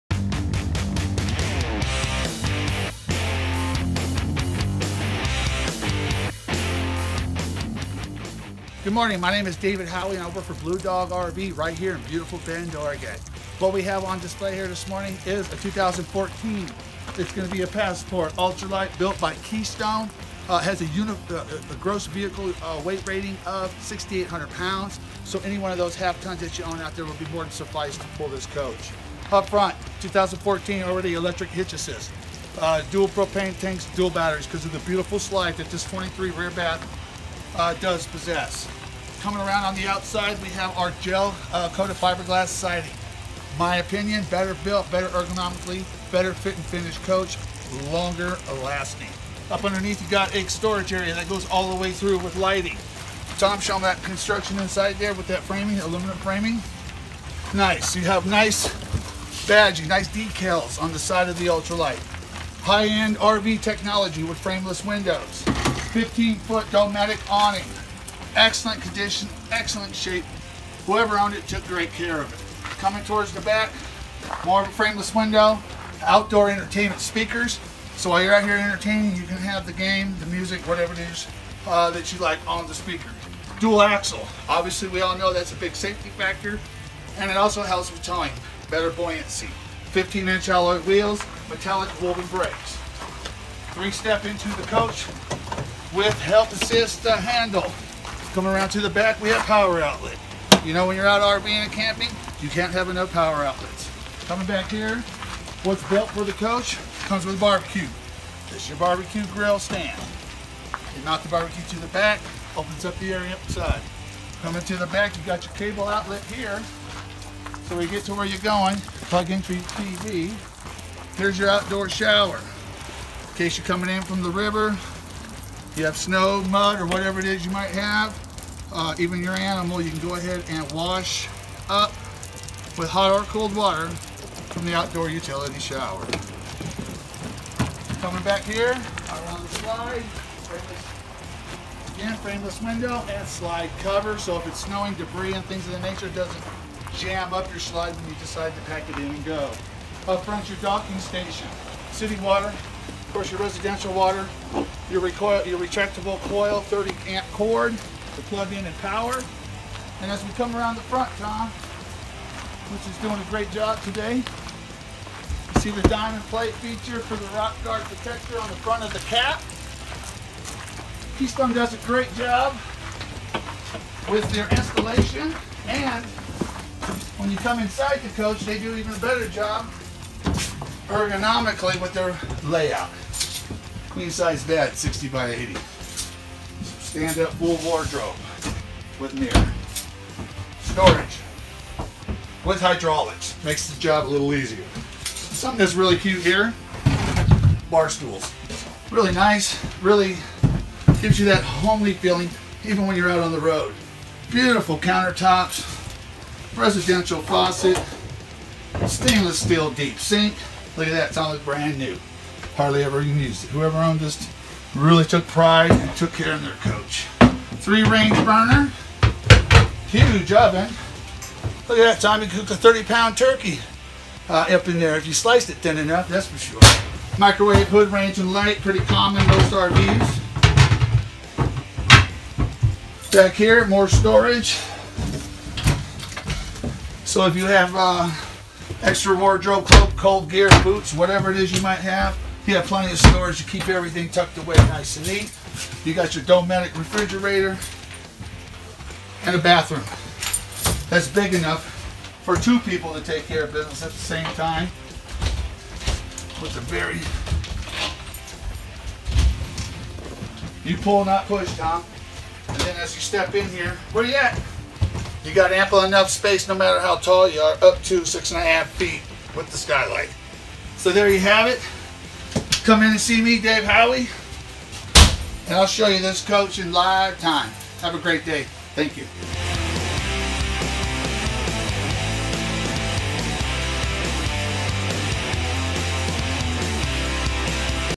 WalkAroundWithDavidHowey
David walks us around an immaculate used 2014 Keystone Passport Ultra Lite 23RB that's sitting on the Bend Blue Dog lot. Trade ins in this great of condition don't come around often, folks! Call David to schedule a VIP appointment to take a look at this stunning Passport and the rest of what we have to offer in our inventory today! :D

Stock#209294A
PRICE: $18,998
SALE PRICE: $16,998
MONTHLY PAYMENTS AS LOW AS $149/mo O.A.C

David Howey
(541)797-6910